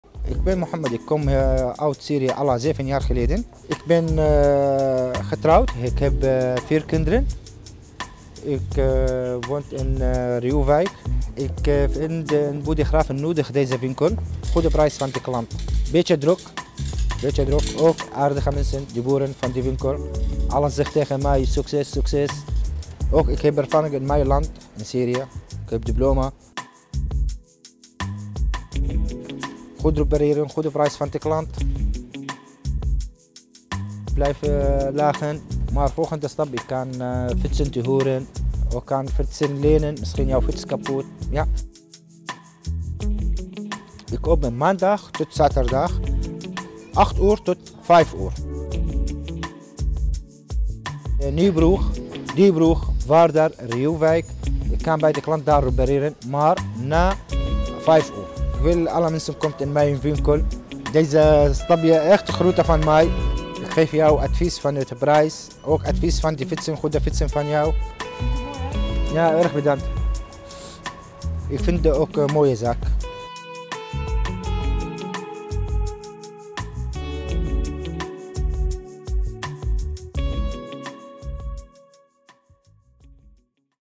TWEEWIELTJES BODEGRAVEN
Nieuwe fietsenwinkel in Bodegraven gerund door Mohammad uit Syrië